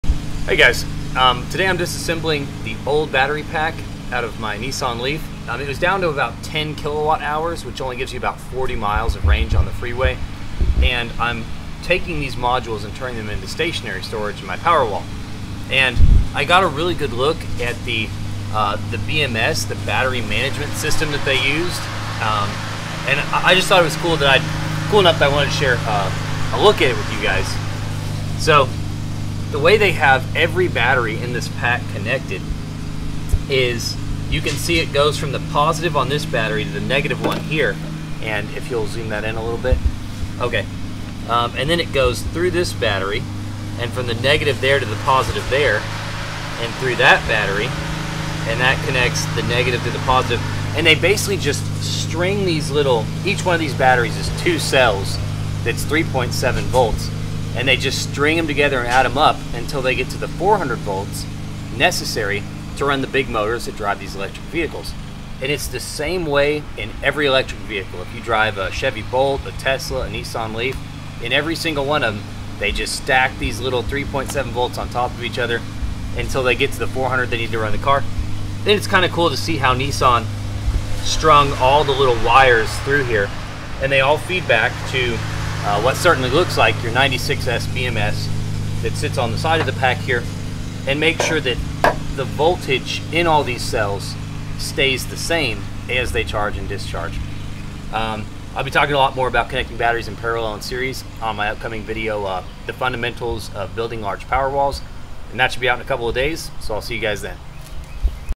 400 volt 96s bms inside an EV
A little glance inside the 24kwh 2010 Nissan Leaf battery pack.  It's capacity has degraded to just over 10kwh.  This limits it's highway range to 35-40 miles, making it difficult to function as a vehicle but perfect for it's next intended use: stationary storage.